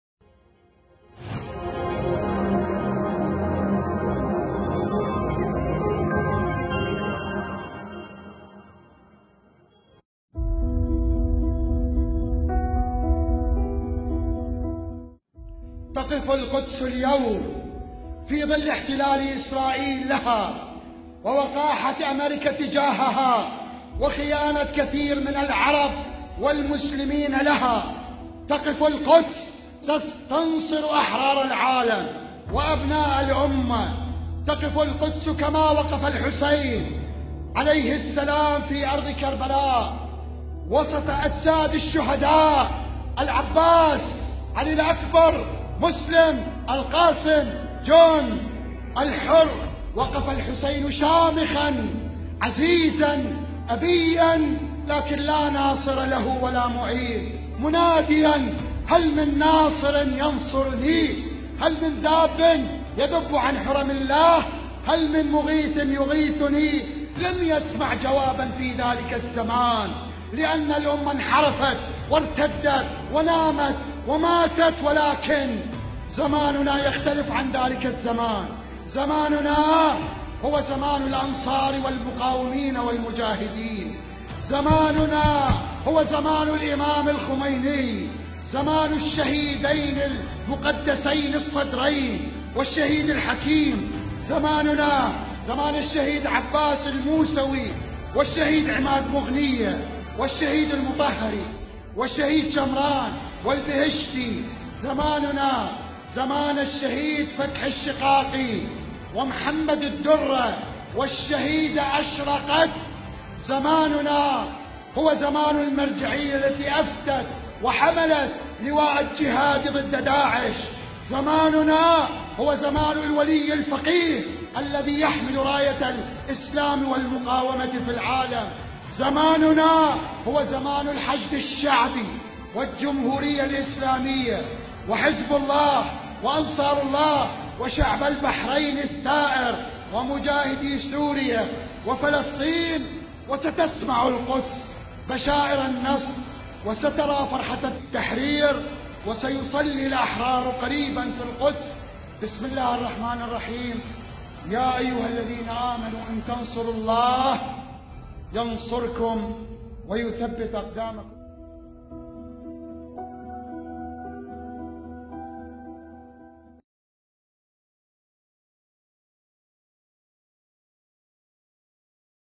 Zaman Kemenangan Al-Quds
Harapan demi harapan kebebasan Al-Quds tiada henti-hentinya, darah mengalir, luka merekah, dan nyawa melayang seakan menyinari asa bangsa Palestina untuk berjuang dari kebinasaan, pengorbanan yang berjalan merupakan harga mahal dari takdir kemenangan.

Al-Quds di ibaratkan laksana Al-Husain yang berdiri sendirian diantara jenazah-jenazah syuhada, di tengah-tengah jasad tak bernyawa beliau memanggil-manggil, masih adakah penolong yang sudi menolongku?, namun tak satupun jawaban terdengar oleh beliau, tapi zaman sekarang sangat berbeda dengan zaman Al-Husain yang terasing.

Zaman ini adalah zaman kemenangan, zamannya Al-Imam Khomeini, zaman kegemilangan Islam yang bangkit di bawah naungan waliyul faqih yang memikul panji Islam dan perlawanan, sehingga teriakan Al-Quds akan disambut sukacita pembebasan dan kabar gembira kemenangan insya Allah.